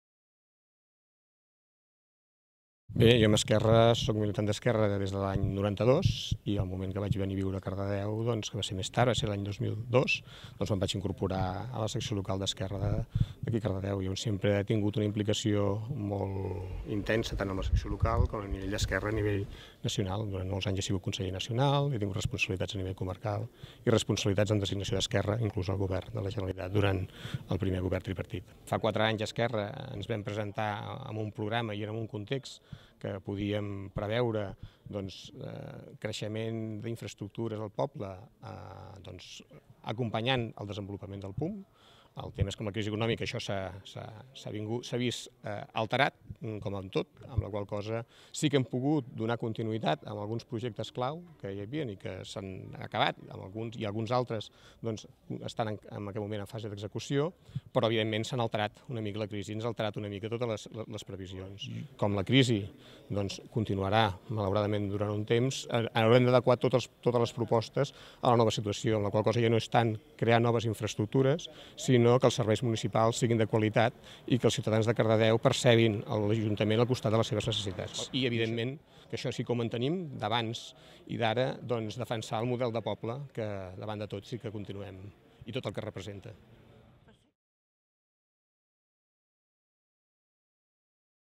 Jordi Cots alcaldable per Esquerra
ERC també ha fet saber ja el seu candidat a alcaldable per les properes eleccions municipals, del maig. Jordi Cots, actual regudor d'urbanisme, serà el proper cap de llista d'aquest partit